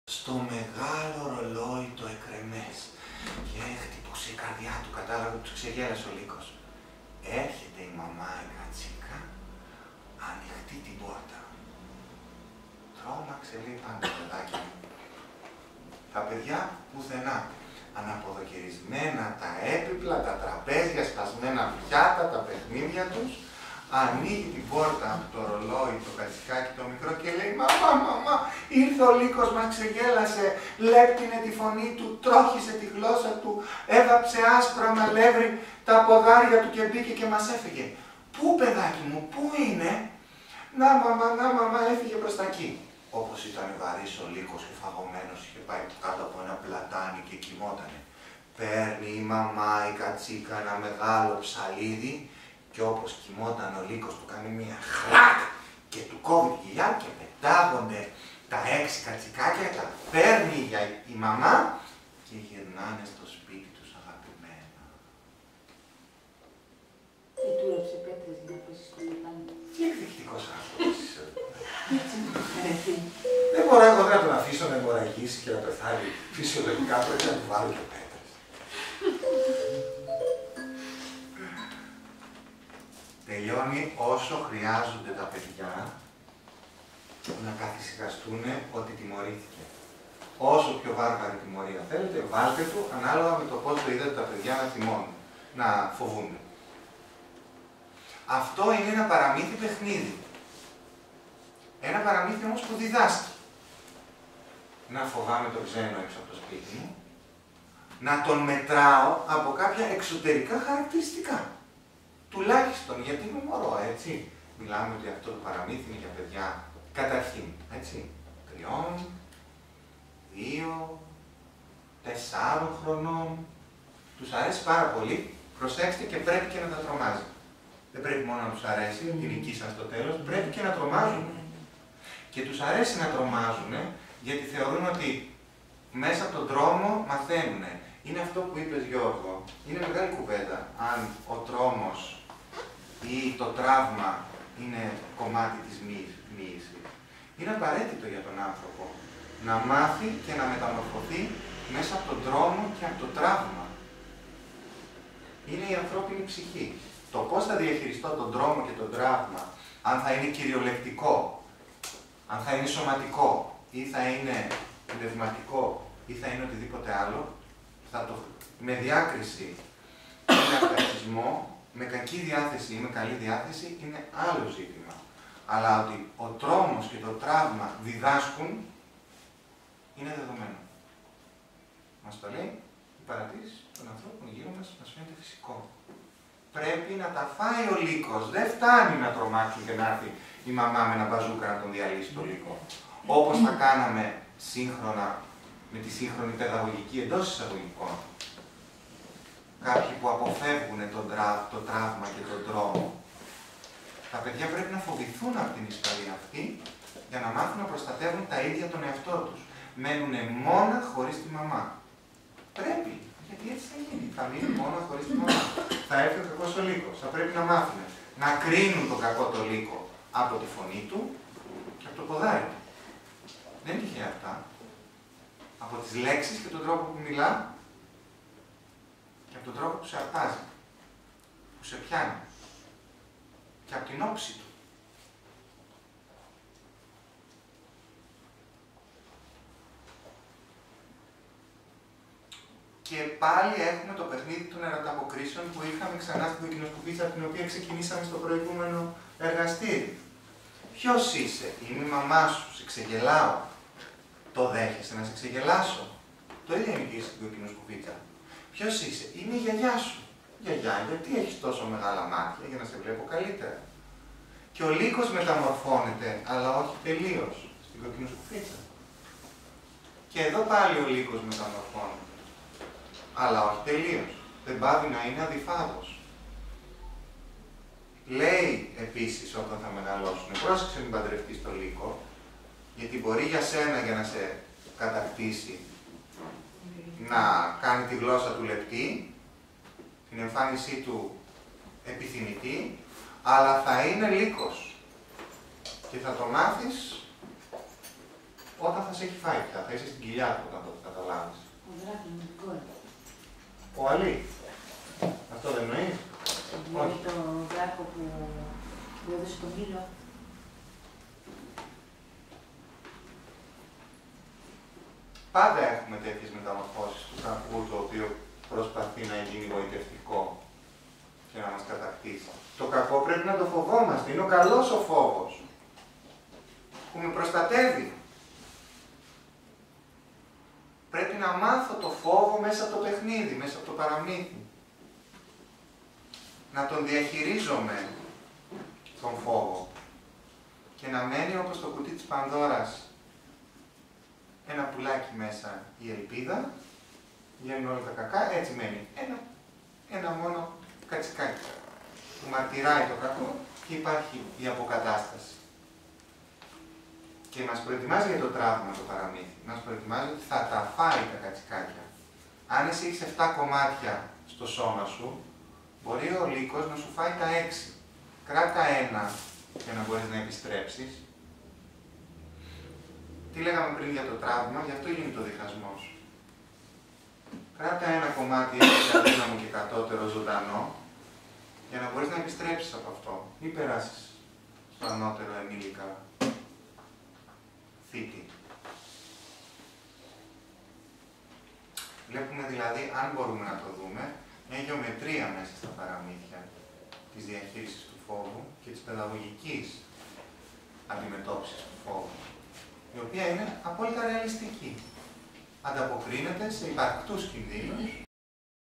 Φόβος και Παραμύθι. Αποσπάσμα σεμινάριου Στέλιου Πελασγού
Απόσπασμα σεμιναρίου του Στέλιου Πελασγού στο Μουσείο Συναισθημάτων για την διαχείριση του φόβου νε οδηγό τα λαϊκά παραμύθια και του μύθους